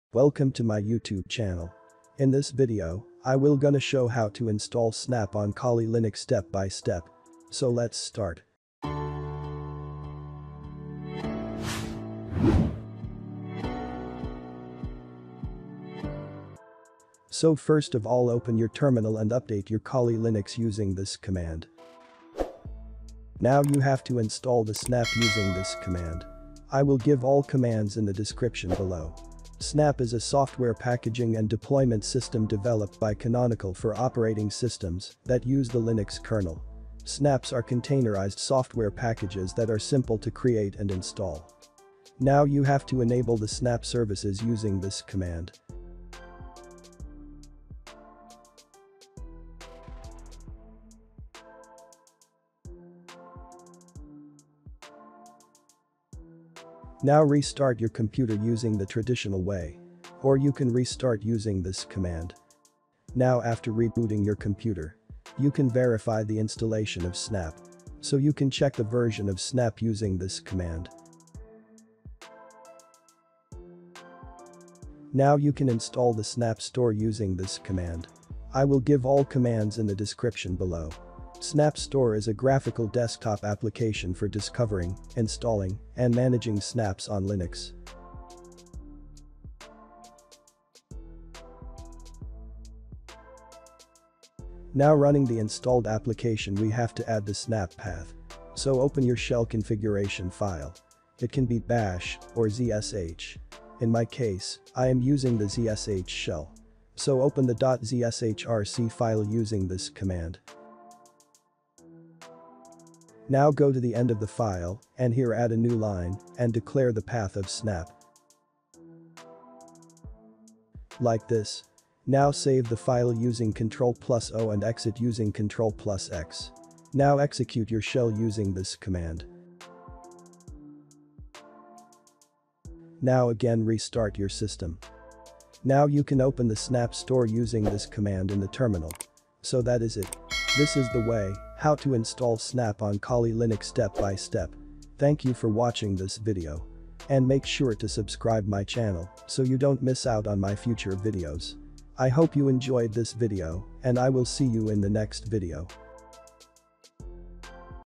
How to snap on Kali Linux | Installing the Snap Store app | Kali Linux 2021.2
In this video I will show How to Install snap on Kali Linux 2021.2 Step by step. Snap is a software packaging and deployment system developed by Canonical for operating systems that use the Linux kernel. Snaps are containerized software packages that are simple to create and install.

️ ☑ How To Install Kali Linux 2021.1

️ ☑ How to Install Kali Linux 2021.2 in VirtualBox on Windows 10

☑ Commands:

$ sudo apt install -y snapd
$ sudo systemctl enable --now snapd apparmor
$ sudo snap install snap-store
$ sudo nano /home/username/.zshrc
       export PATH=$PATH:/snap/bin
$ source /home/username/.zshrc
$ init 6

That is it! This is the Way How to Install snap on Kali Linux 2021.2 Step by step.